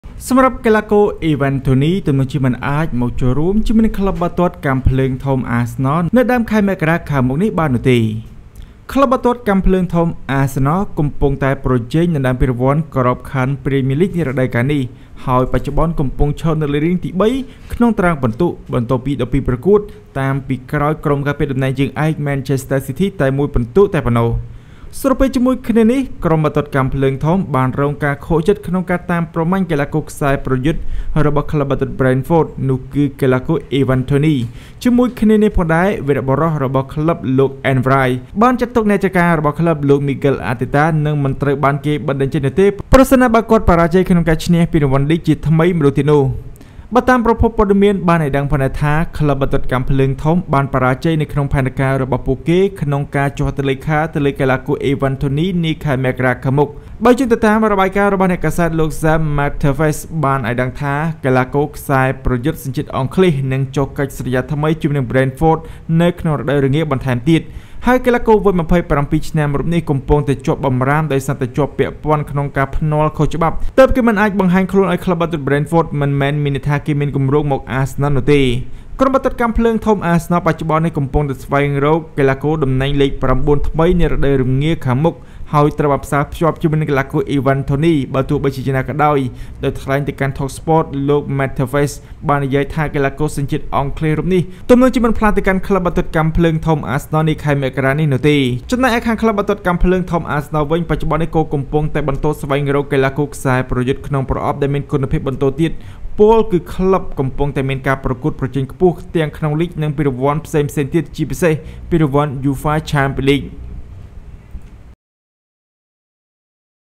Ivan Toney ទំនងជាមិនអាចមកចូលរួមជាមួយក្រុមកាំភ្លើងធំ Arsenal ដើមខែមករាខាងមុខនេះបានទេ
១.Ivan Toney ទំនងជាមិនអាចមកចូលរួមជាមួយក្រុមកាំភ្លើងធំ Arsenal ដើមខែមករាខាងមុខនេះបានទេ